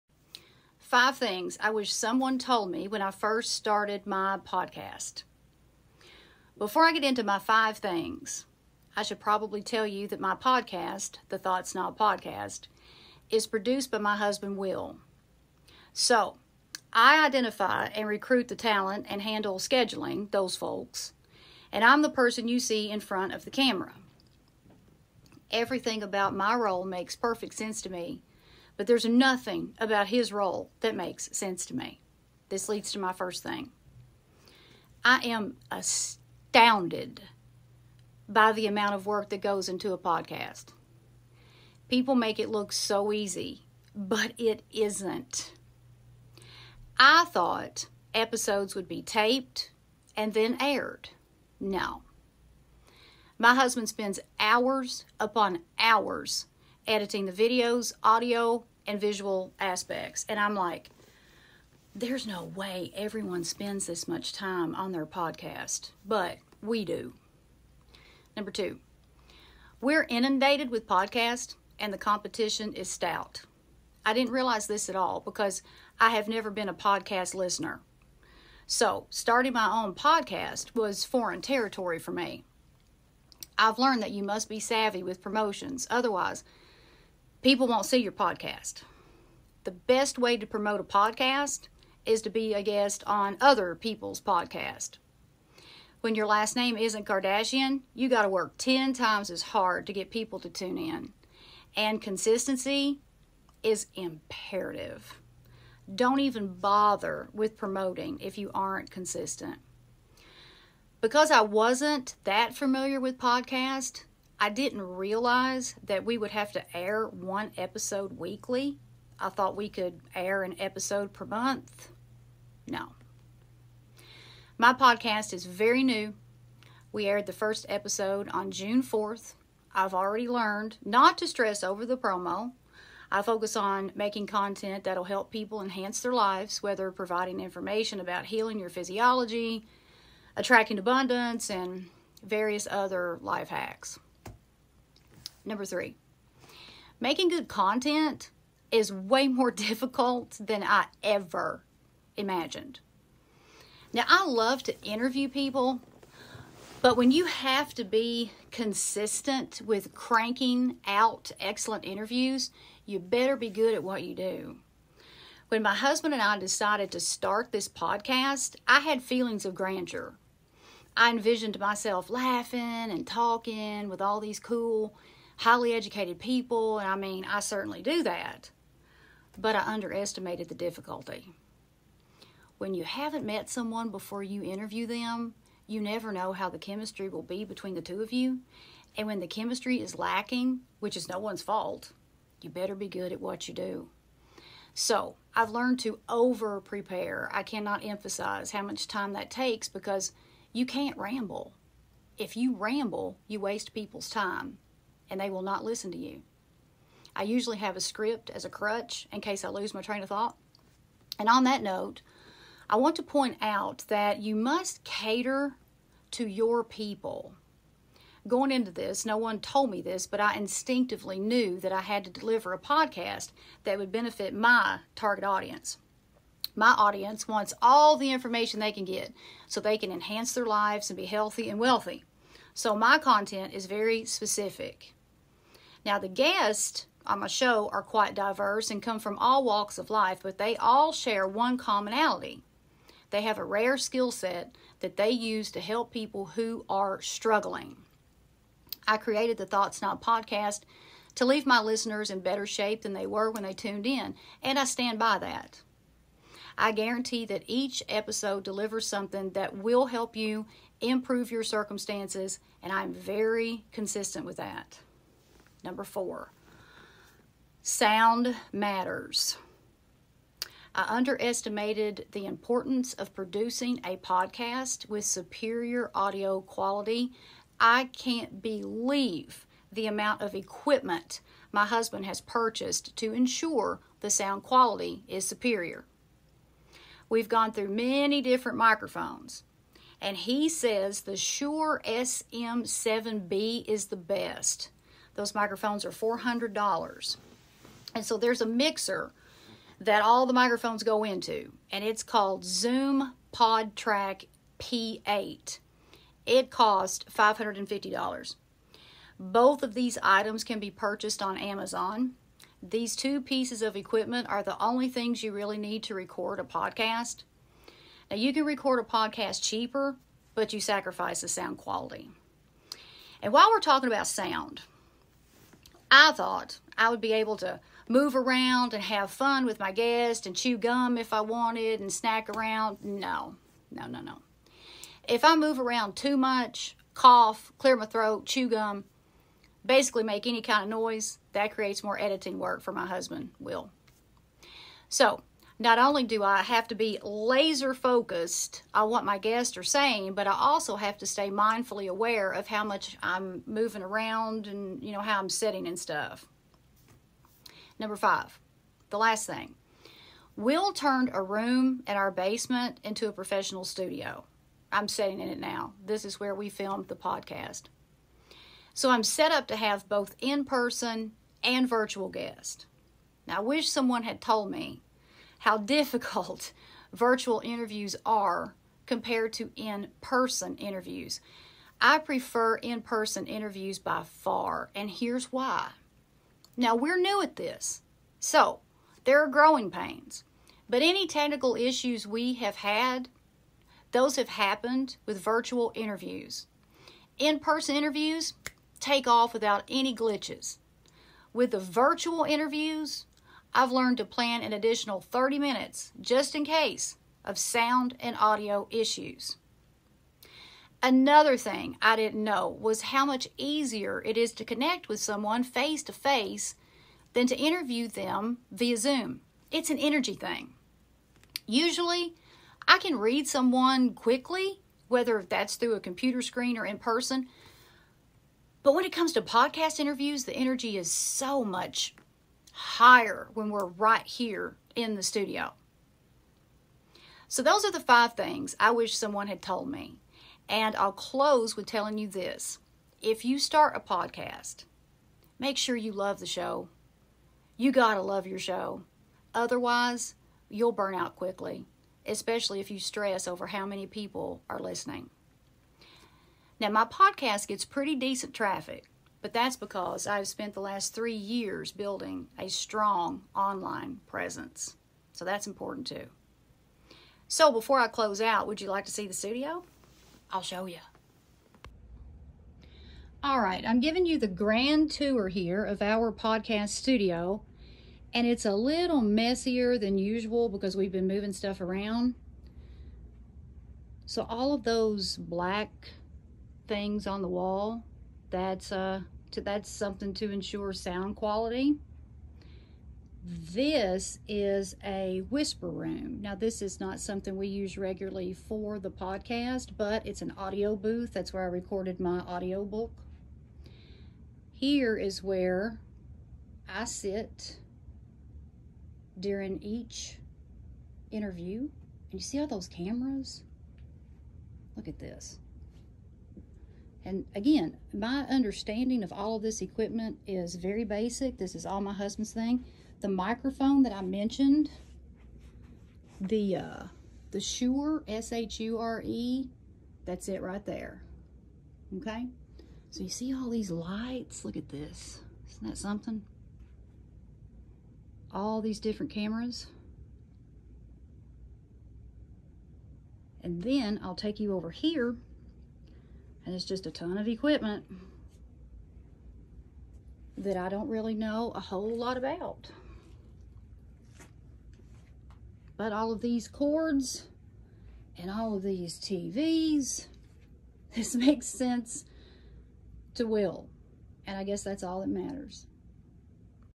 5 Things I Wish Someone Told Me When I First Started a Podcast
Podcasts are a tremendous amount of work, and I wish I had known about these 5 things before I started my podcast.